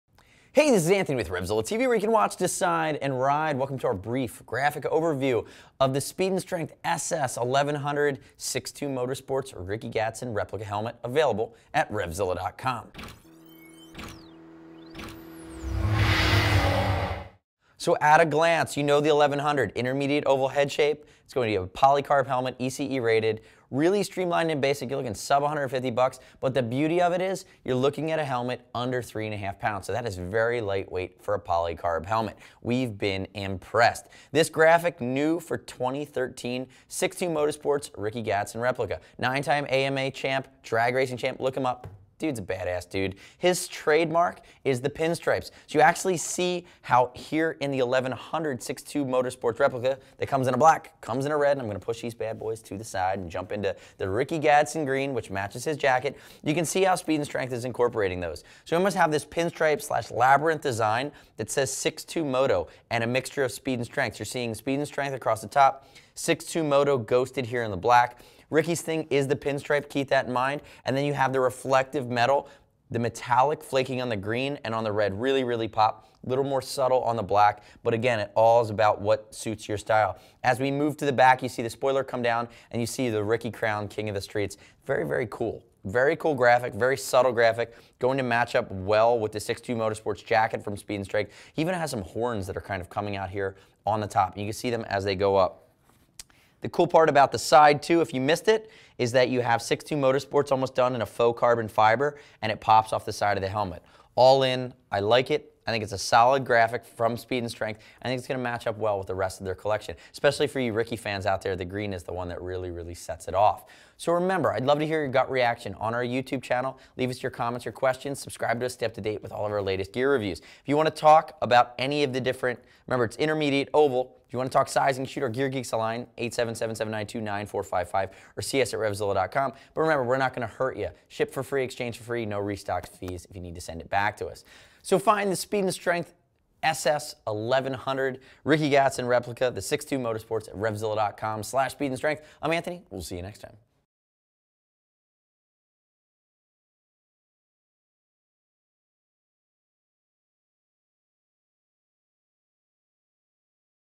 Speed & Strength SS1100 62 Motorsports Helmet Review at RevZilla.com
Speed & Strength SS1100 62 Motorsports Helmet Review
The Speed & Strength SS1100 62 Motorsports feature the Ricky Gadson classic pinstripe style and is a perfect mate to the 62 Motorsports jacket.The 62 Motorsports graphic comes in Black, Red or Green, which are all showcased in this video. For a detailed breakdown on the SS1100 helmet, check out this video